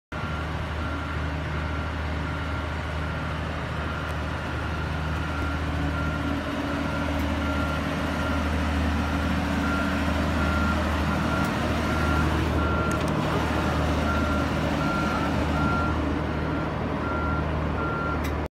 IE 29000 Class DMU (29101) at Maynooth Station
Just taught I’d do a short video of opening the door on 29101 at Maynooth